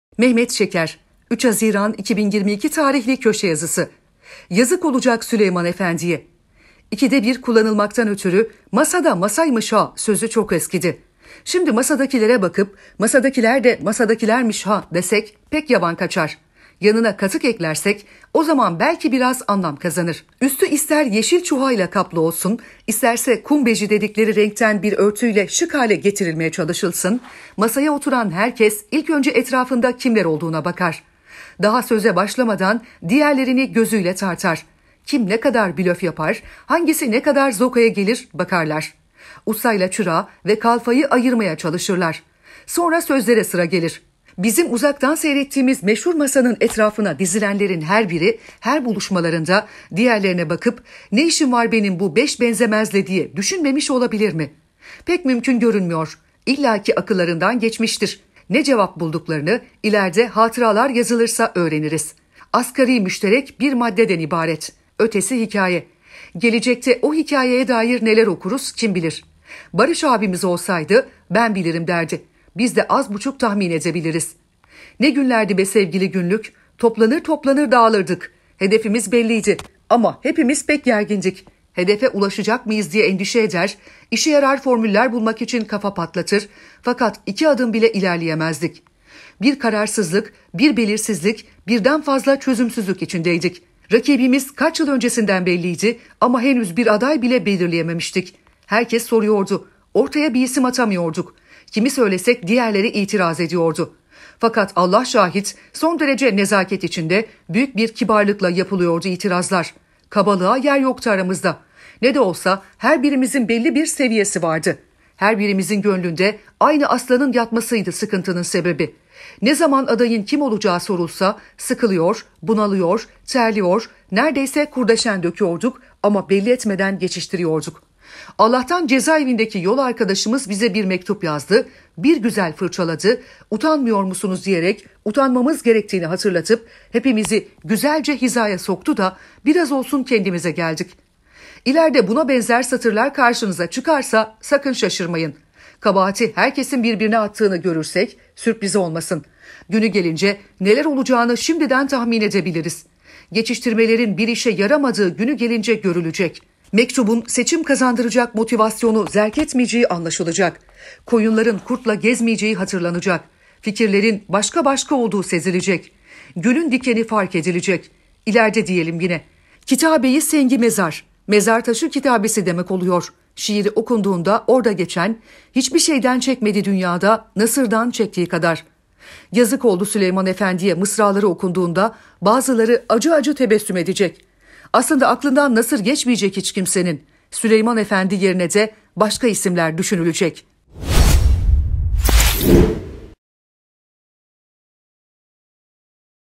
YAZIK OLACAK SÜLEYMAN EFENDİ'YE
İkide bir kullanılmaktan ötürü “Masa da masaymış ha” sözü çok eskidi. Şimdi, masadakilere bakıp “masadakiler de masadakilermiş ha” desek pek yavan kaçar. Yanına katık eklersek, o zaman belki biraz anlam kazanır. Üstü ister yeşil çuhayla kaplı olsun, isterse kum beji dedikleri renkten bir örtüyle şık hâle getirilmeye çalışılsın, masaya oturan herkes, ilk önce etrafında kimler olduğuna bakar. Daha söze başlamadan, diğerlerini gözüyle tartar.